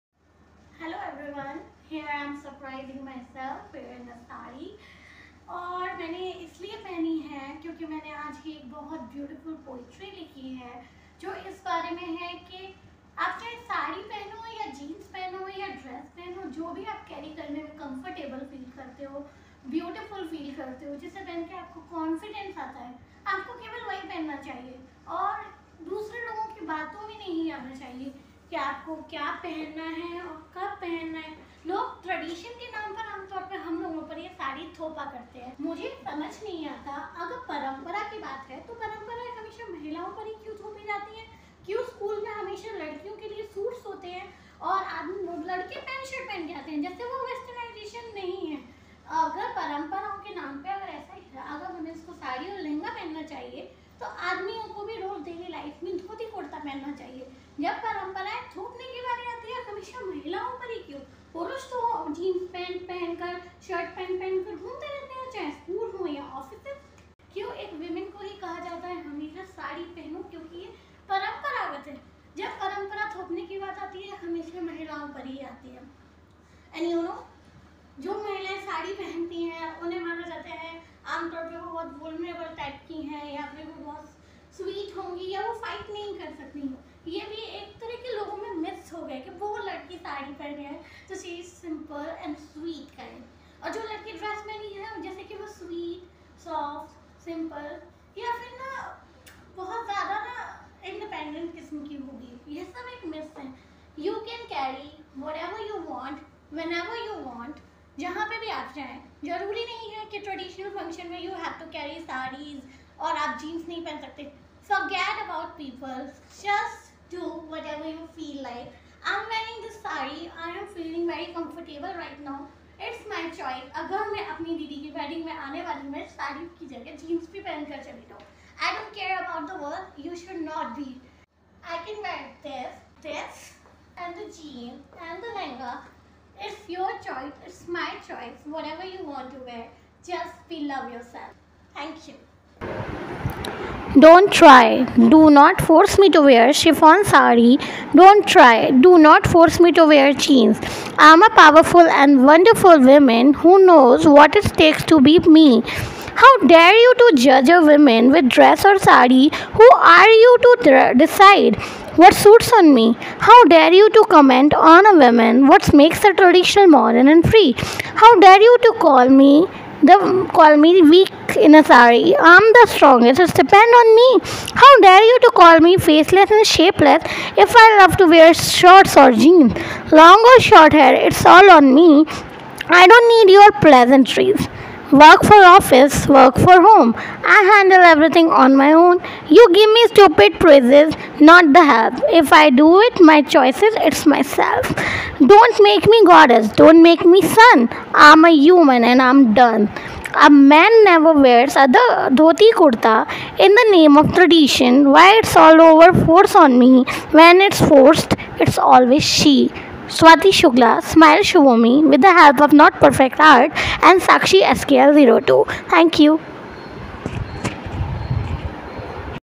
'She' : Inspiring Poem By Me.
'She' is an inspiring poem by me , Swati Shukla based on the facts of our society. it questions the thinking of people who think womens who wear specific clothings do specific things and thus , they should wear a specific clothing and be limited to a specific thing.

The whole reason why I wore a Saree for the first time in my life is this poem I wrote today ― 2 Sep 2021!!!

If you like it, please like it & share it. you will be very appreciated!